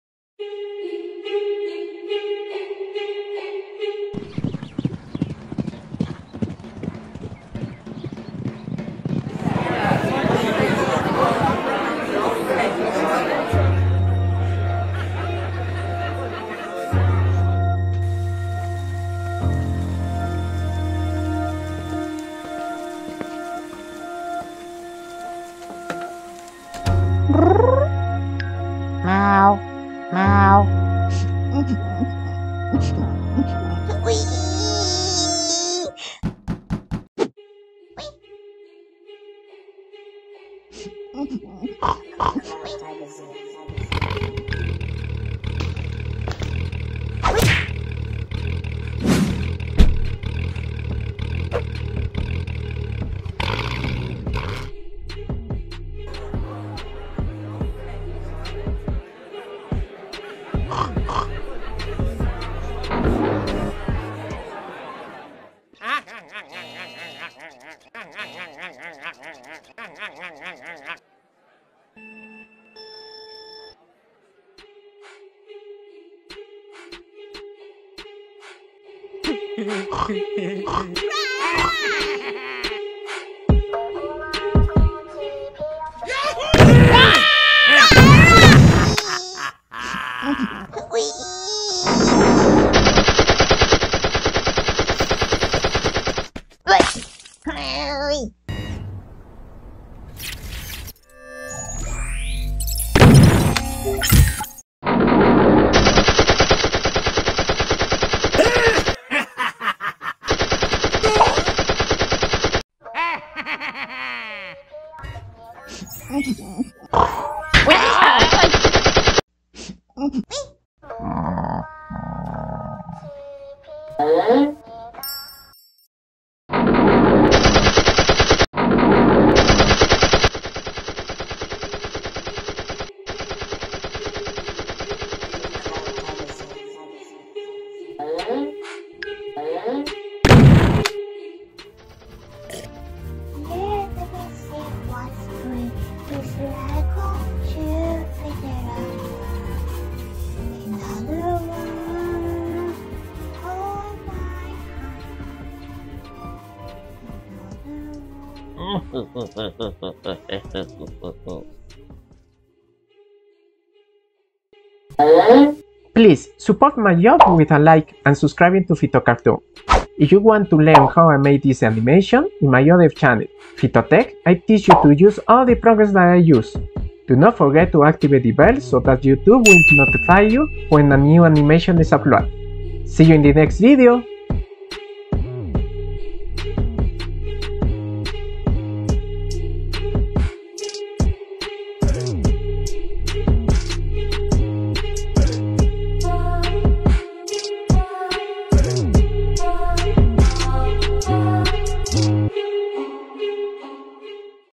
a SQUID GAME animation
SQUID-GAME fan Animation

I made this animation using Adobe Animate in Frame by frame style, so it takes me more than a month to finish it, I hope you like it